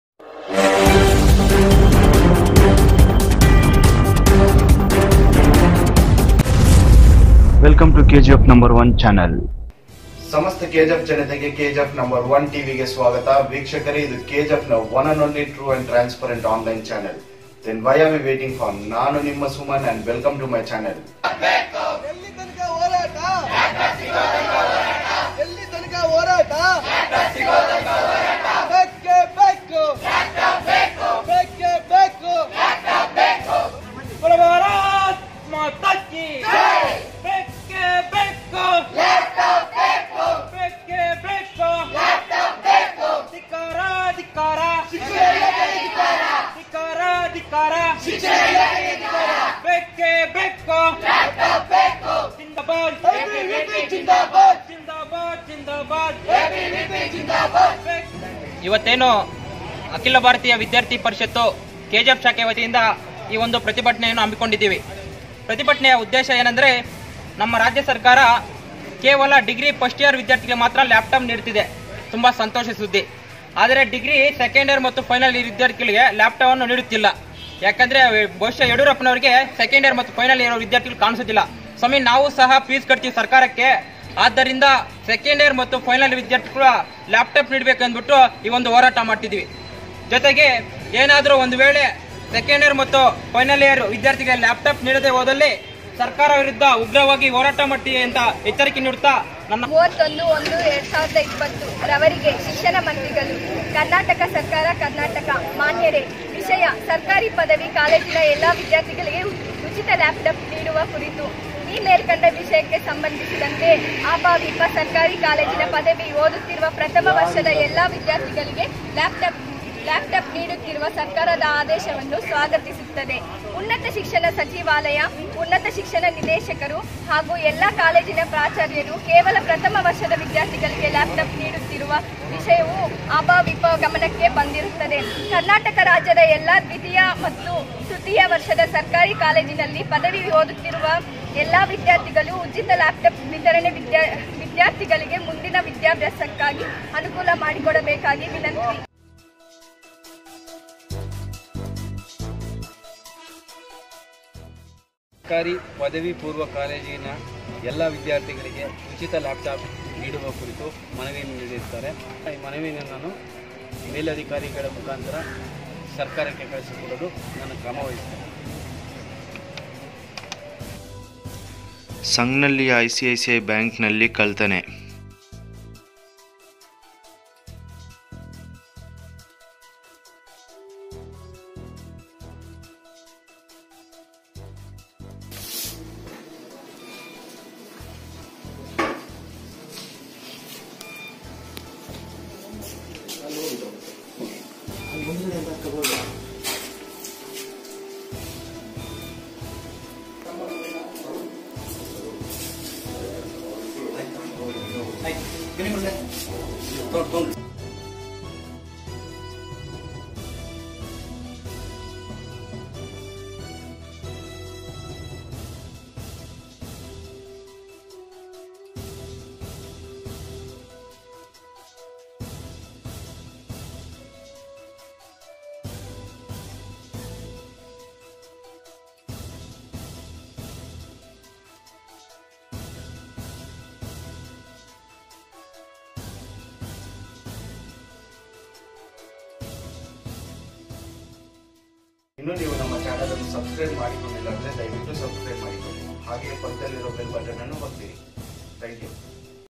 KGFNO1TV | ABVP TEAM GIVEN MEMORANDUM TO TAHSILDHAR | SANGANAHALLI ICICI BANK NEWS
KGF TALUK ONE AND ONLY TRUE & TRANSPARENT NEWS CHANNEL.